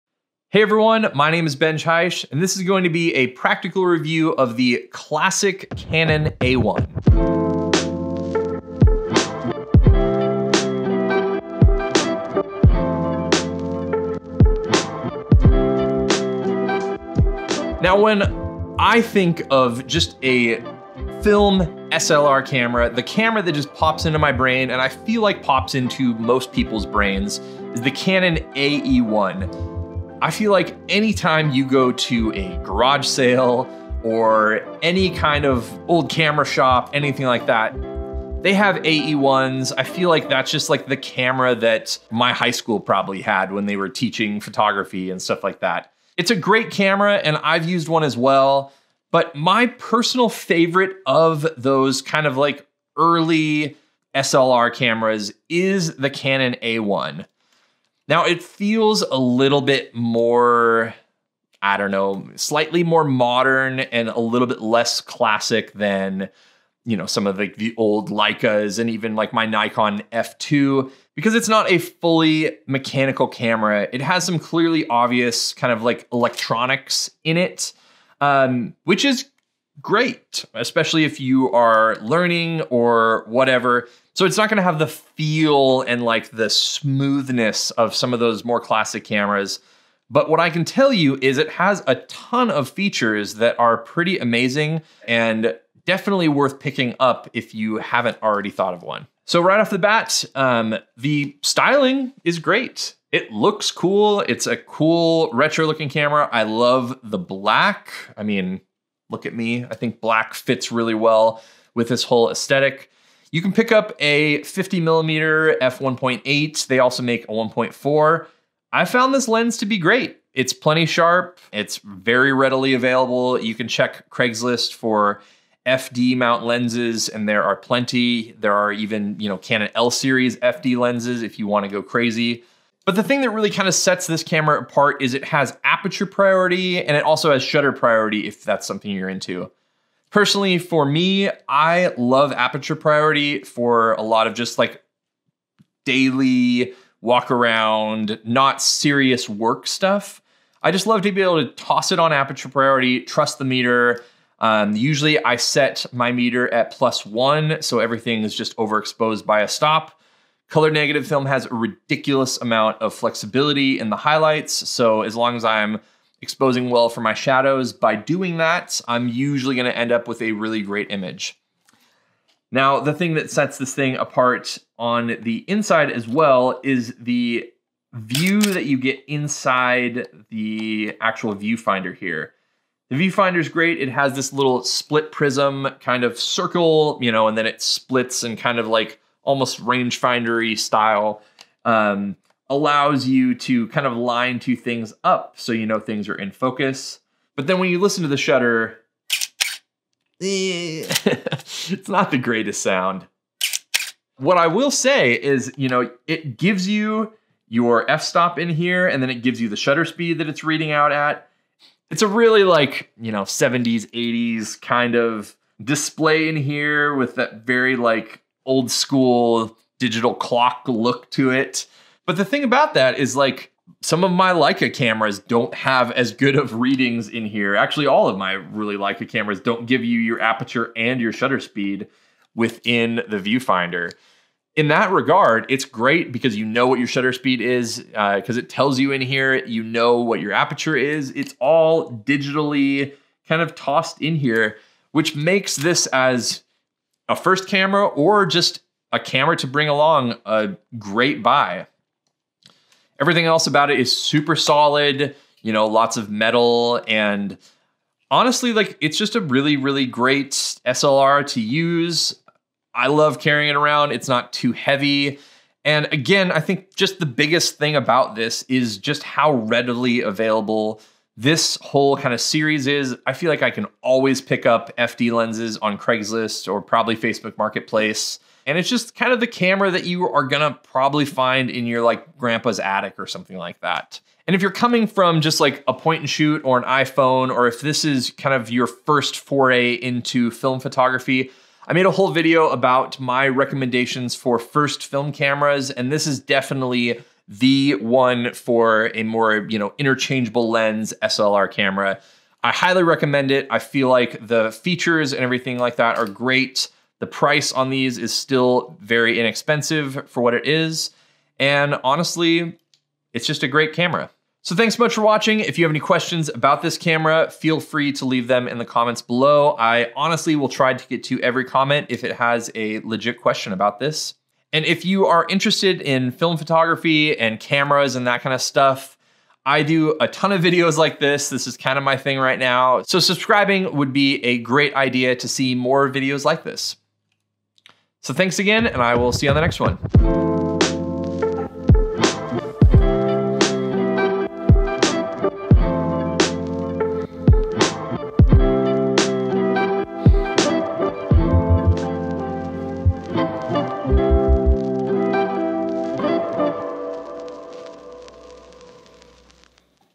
Canon A-1 Film Camera | Practical Review
The Canon A1 is my favorite beginner camera for those looking for a more “classic” film SLR. It packs so many features and out performs the AE-1 as well in my opinion.

My filming gear:
Main Camera
Studio Wide Lens
Standard Lens
2nd Camera
Shotgun Mic
Budget Video mic
Podcast Mic
Studio Light